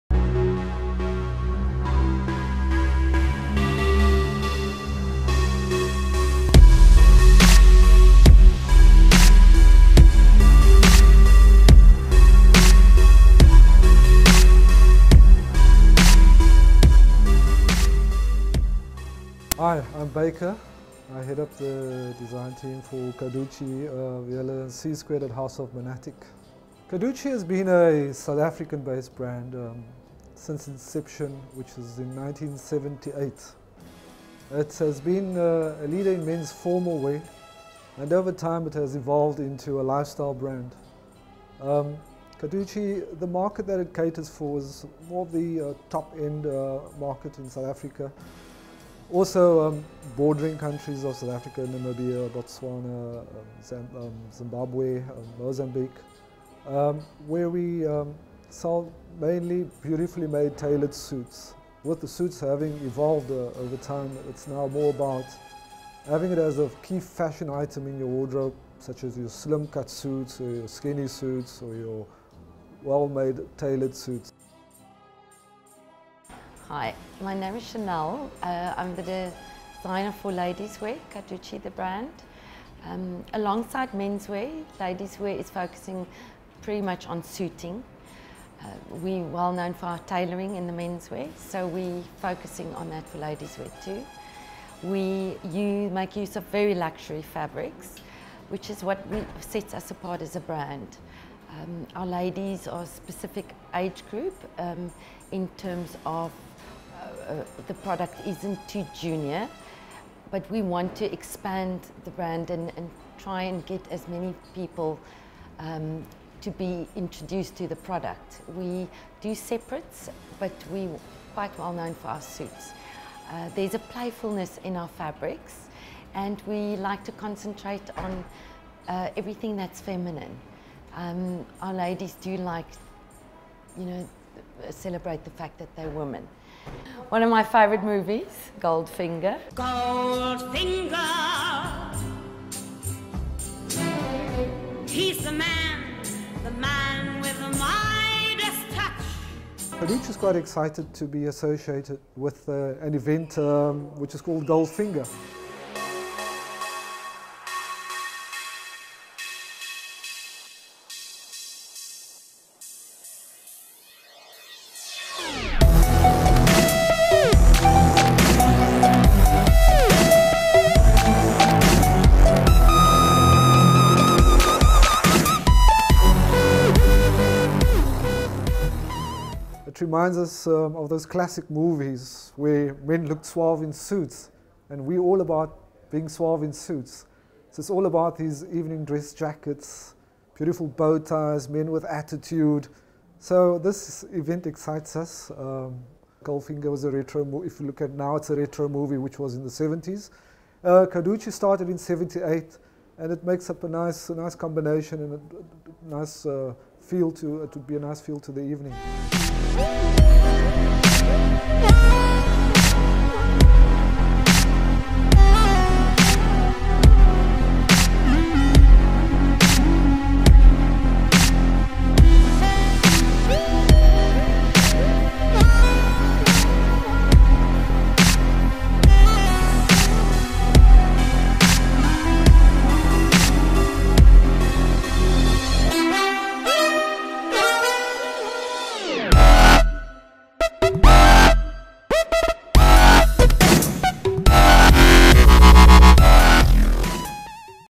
Carducci Fashion Walk ( THE GOLDFINGER )
The Goldfinger Fashion Walk is a theme gathering of socialites and fashion influencers in South Africa.  It featured an exclusive theatrical fashion show by Carducci and CSquared of "House of Monatic" at the most luxurious Night Club in the city of Cape Town The Bank".

The fashion show was directed by the iconic Jan Malan who is a renowned international fashion show producer / director and the founder of Umzingeli Productions.
The Goldfinger Theatrical Fashion Walk was conceptualized by Jimisterio Catwalk Academy and the video content was produced by Jimisterio Productions. The Jimisterio Catwalk Academy insists there is great potential for a sustaining platform through theatrical fashion shows and fashion films worldwide.